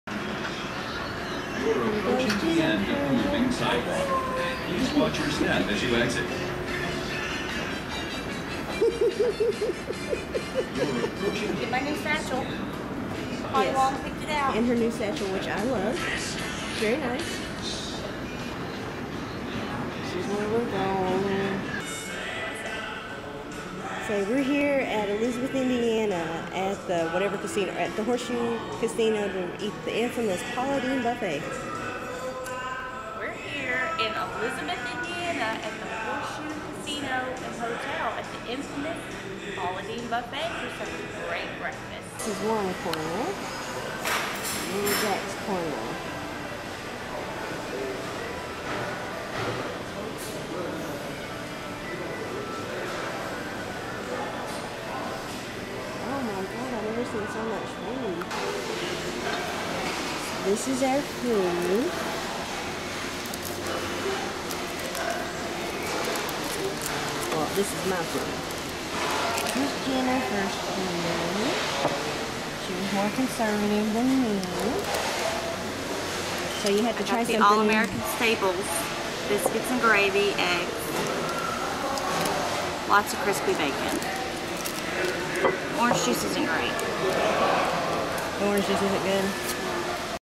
Connie and Jennafer's Food Vlog #1: Paula Deen's
Connie and bff Jennafer travel to Elizabeth, Indiana to the Horseshoe Casino to sample the savory southern cooking sensation that is Paula Deen.